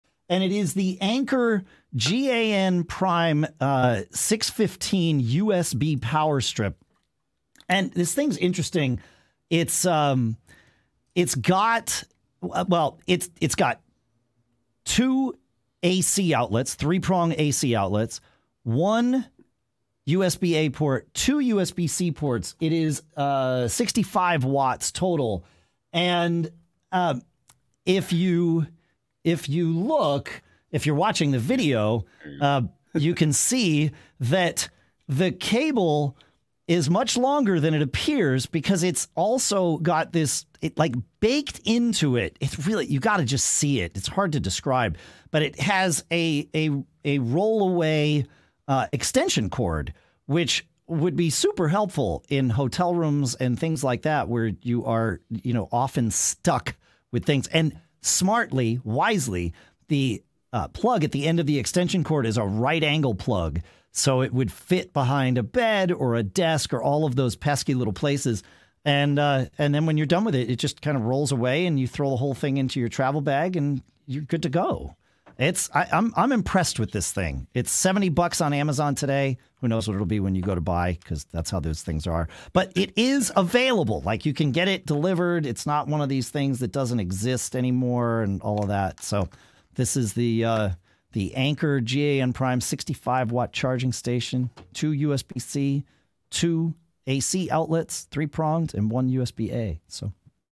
Anker GanPrime 65W Charging Station
Here's a great Power Strip from Anker that's great for travel and work!